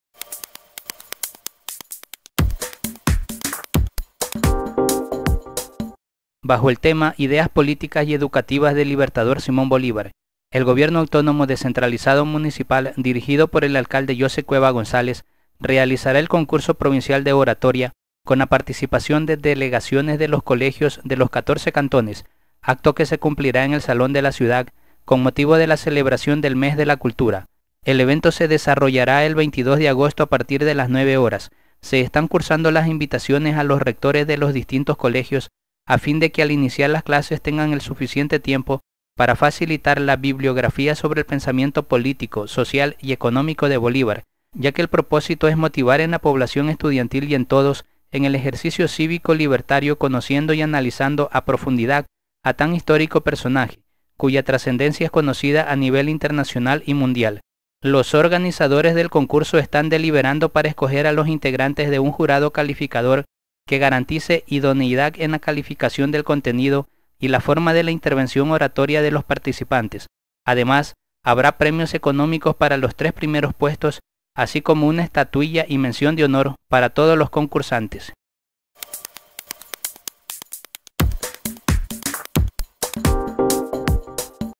Se realizará concurso provincial de oratoria
Bajo el tema "IDEAS POLÍTICAS Y EDUCATIVAS DEL LIBERTADOR SIMÓN BOLÍVAR" el Gobierno Autónomo Descentralizado Municipal dirigido por el Alcalde Joseph Cueva González realizará el Concurso Provincial de Oratoria con la participación de delegaciones de los colegios de los catorce cantones